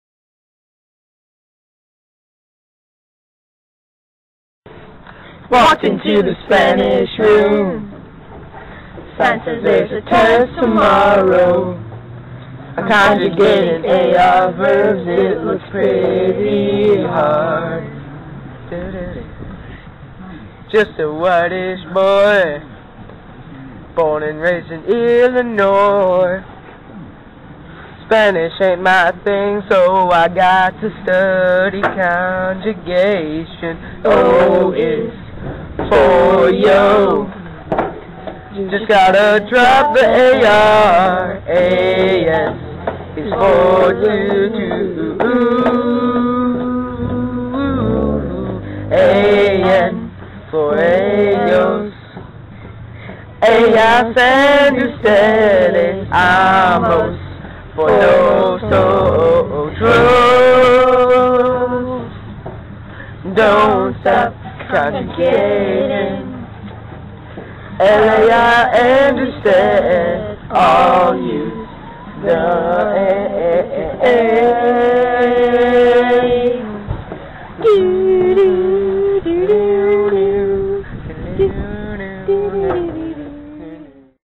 Don't Stop Conjugating!
Ash and Jess, along with special guests Peter and Joaquin made our spanish project on Conjugating -AR verbs.  Pretty amazing if I do say so myself :) We all used our spanish names at the end, in case you all are wondering.